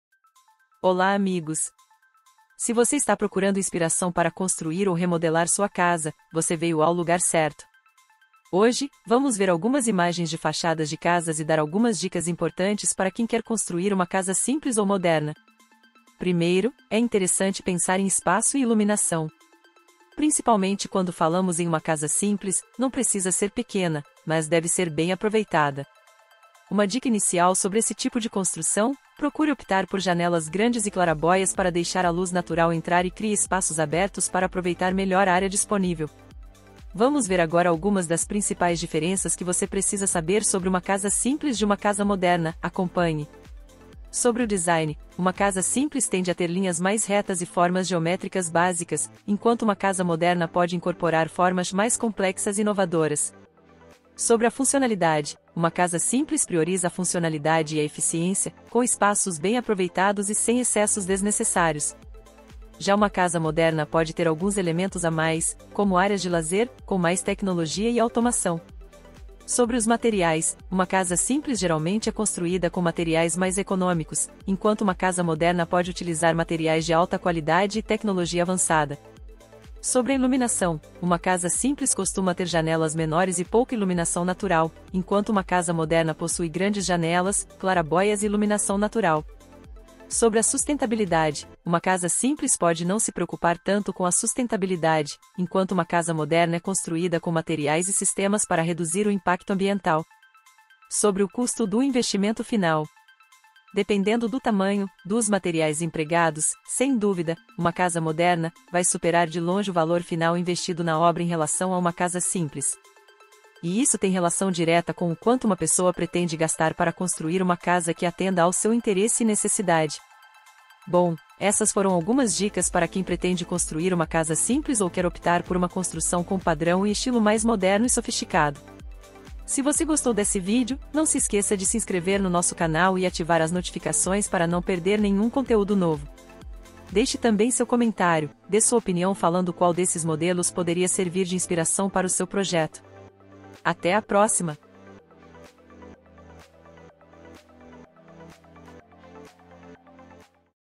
Sua Casa dos Sonhos: Dicas para Construir com Estilo (VEJA AGORA!)💒
Olá, amigos! Se você está procurando inspiração para construir ou remodelar sua casa, você veio ao lugar certo. Hoje, vamos ver algumas imagens de fachadas de casas e dar algumas dicas importantes para quem quer construir uma casa simples ou moderna.

Primeiro, é interessante pensar em espaço e iluminação. Principalmente quando falamos em uma casa simples, não precisa ser pequena, mas deve ser bem aproveitada. Uma dica inicial sobre esse tipo de construção, procure optar por janelas grandes e clarabóias para deixar a luz natural entrar e crie espaços abertos para aproveitar melhor a área disponível.

Vamos ver agora algumas das principais diferenças que você precisa saber sobre uma casa simples de uma casa moderna, acompanhe:

Sobre o Design: Uma casa simples tende a ter linhas mais retas e formas geométricas básicas, enquanto uma casa moderna pode incorporar formas mais complexas e inovadoras.

Sobre a Funcionalidade: Uma casa simples prioriza a funcionalidade e a eficiência, com espaços bem aproveitados e sem excessos desnecessários. Já uma casa moderna pode ter alguns elementos a mais, como áreas de lazer, com mais tecnologia e automação.

👍VEJA O CONTEÚDO COMPLETO NO VÍDEO!

Deixe também seu comentário, sua opinião falando qual desses modelos poderia servir de inspiração para o seu projeto.